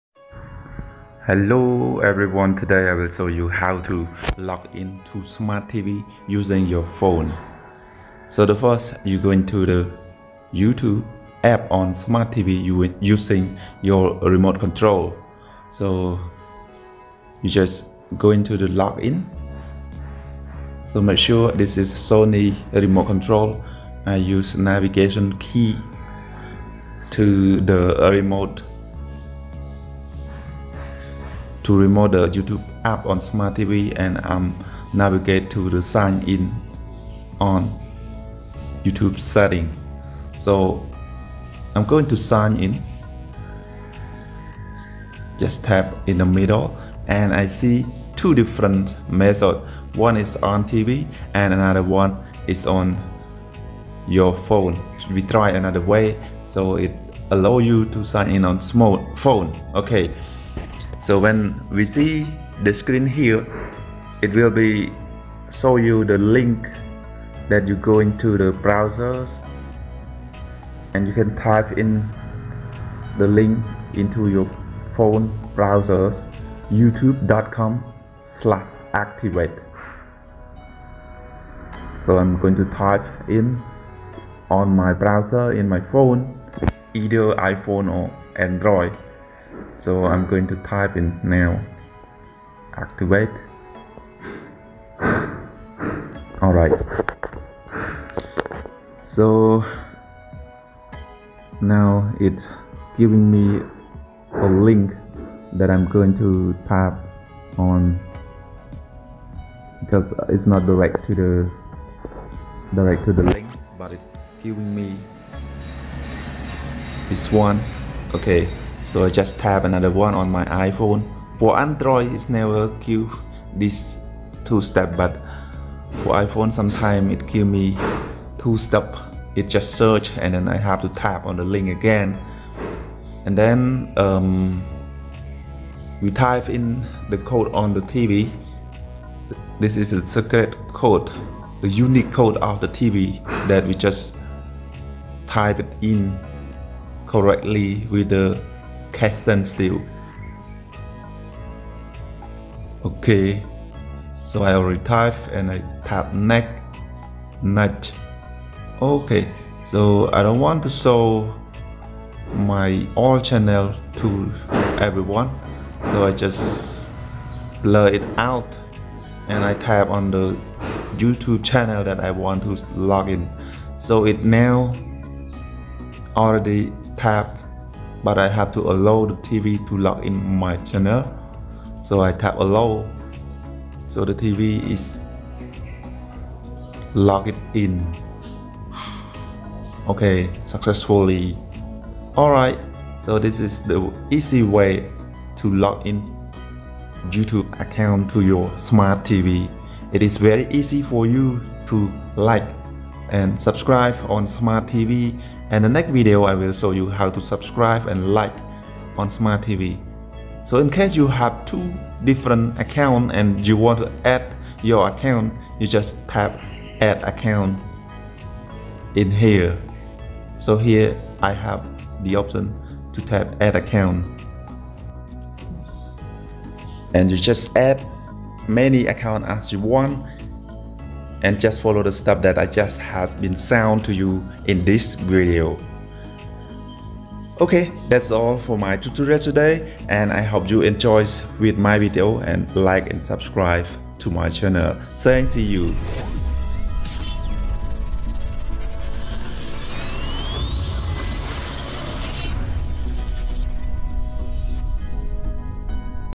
How to sign in YouTube on Smart TV / log in YouTube channel account on Smart TV with a smart phone
this video will show you how to make free energy water pump from the drum